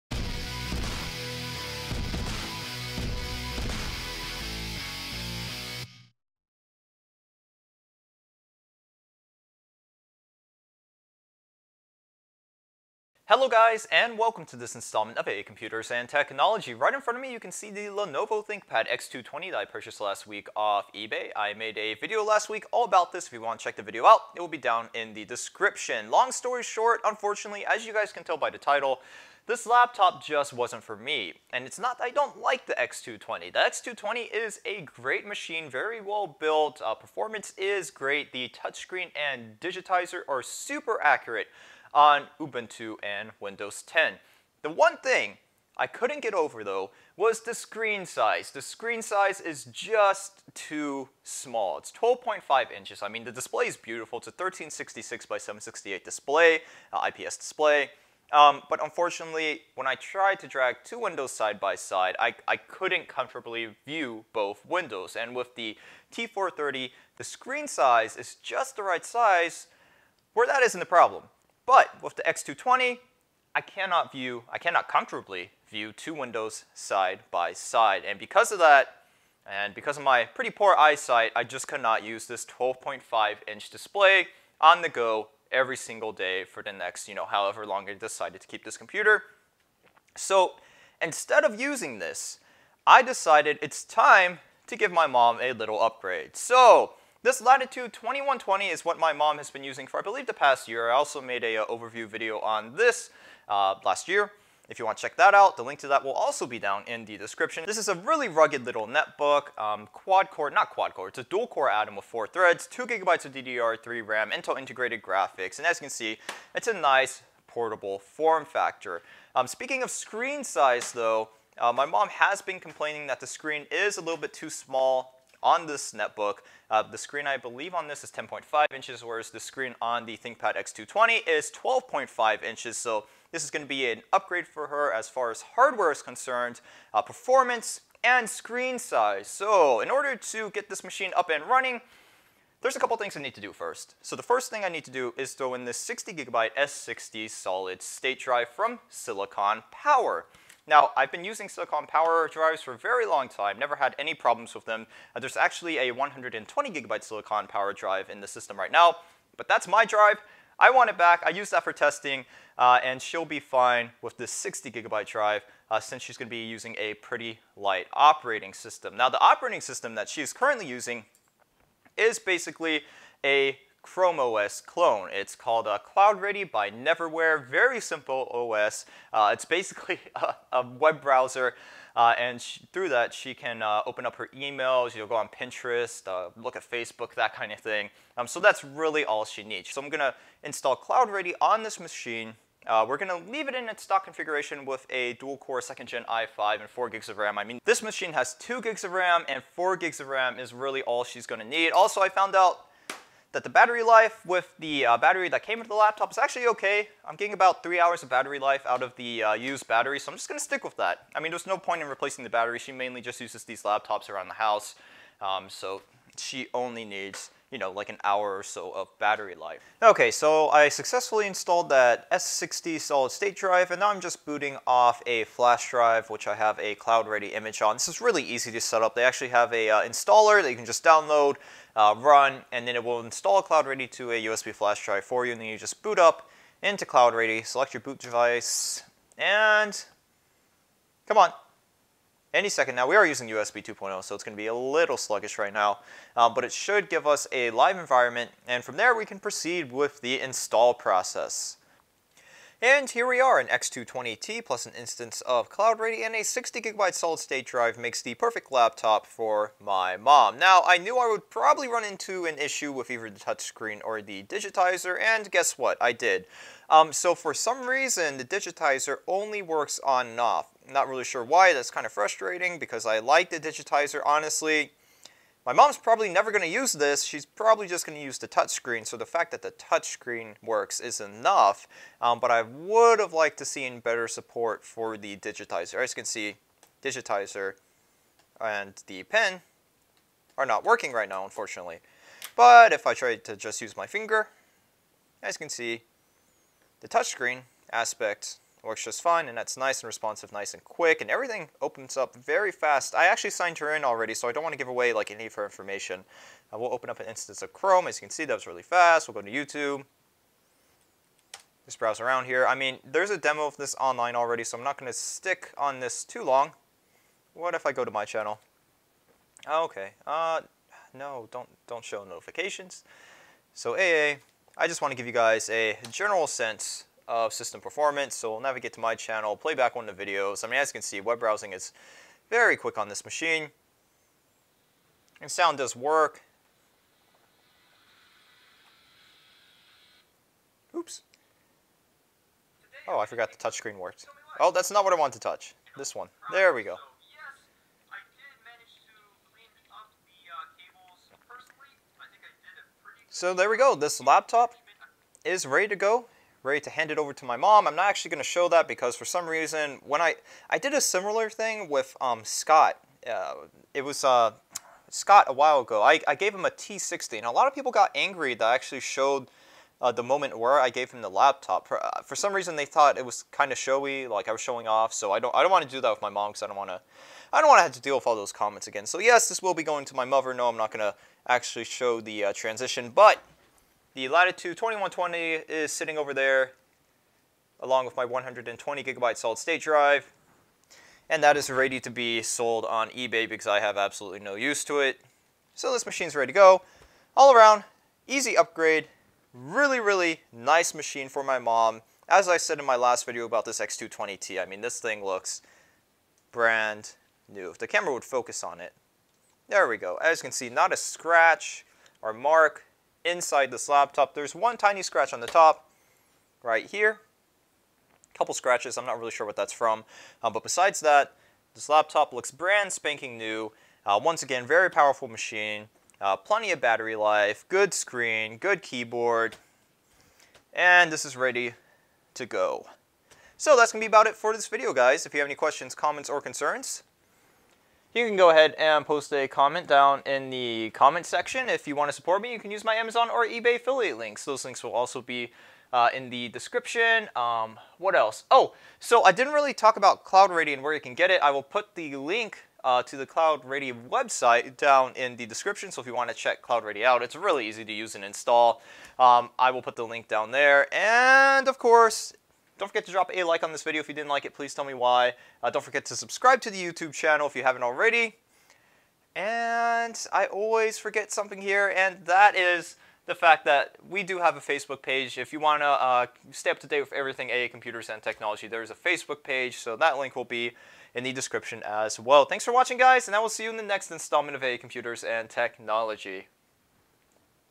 Setting up the Perfect Laptop for Mom: Thinkpad X220T, SSD, Cloudready
It turns out the X220 was just not for me.  It's a great laptop, well built, plenty of horsepower, good battery life, and lots of great features.  However, I found the 12.5" display to be a little too small for my personal preference.

Instead of keeping the X220t for myself, I have decided to give it for my mom.  I have a little bit of work to do before it is ready for her to use.  In this video I will be installing a 60GB SSD and cloudready on the X220t in preparation for handing the laptop over.

Silicon Power 60GB SSD ($32)

Support us!